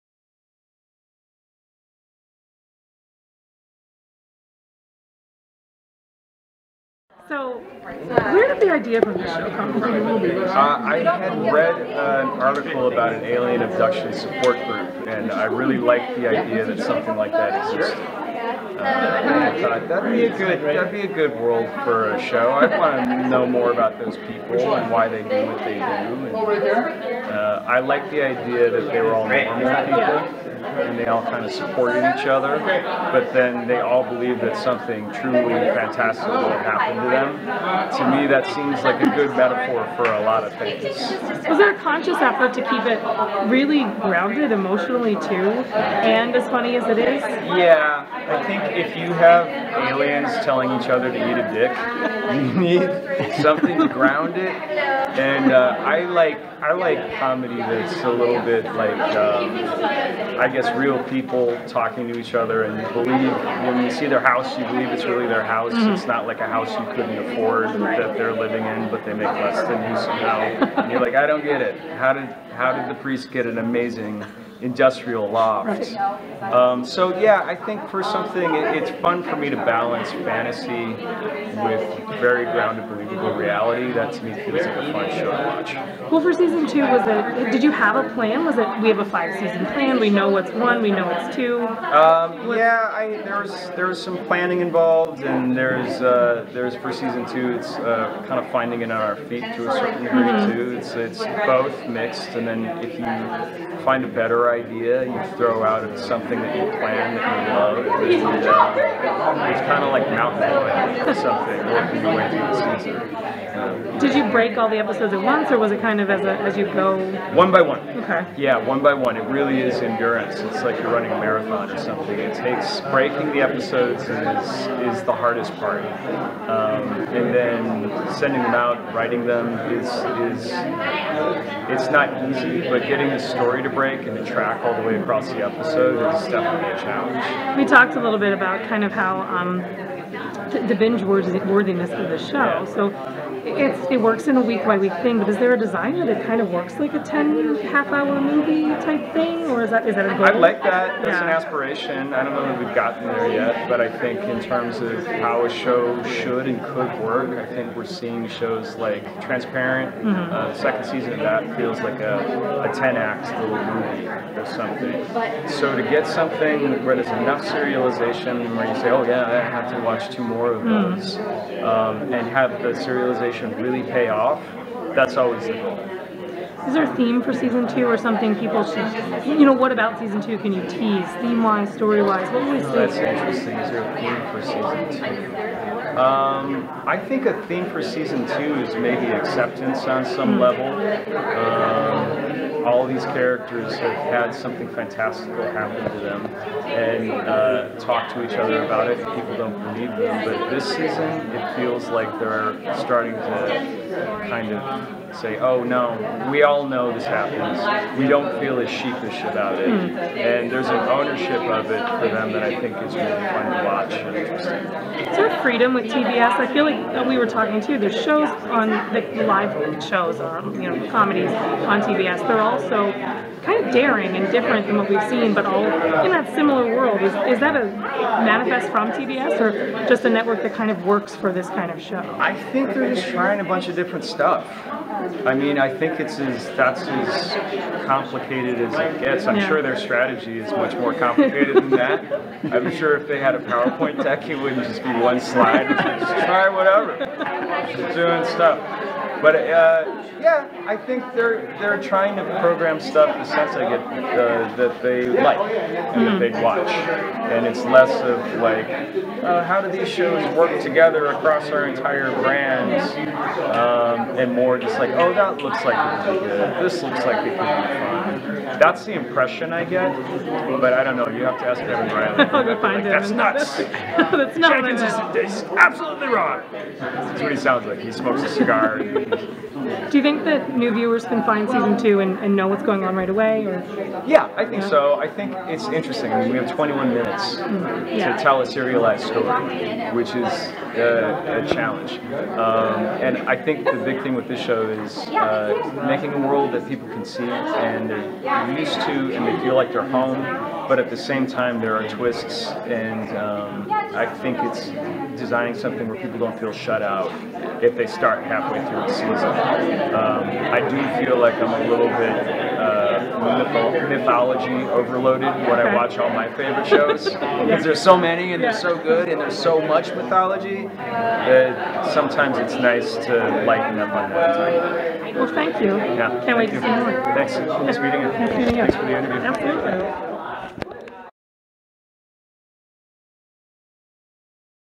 Interview With tbs' People of Earth Creator & EP David Jenkins at Comic-Con 2017
Creator, executive producer and writer David Jenkins of tbs' People of Earth talks about season two of the show, where the original idea came from, plan for the series, serialization, and more at Comic-Con 2017.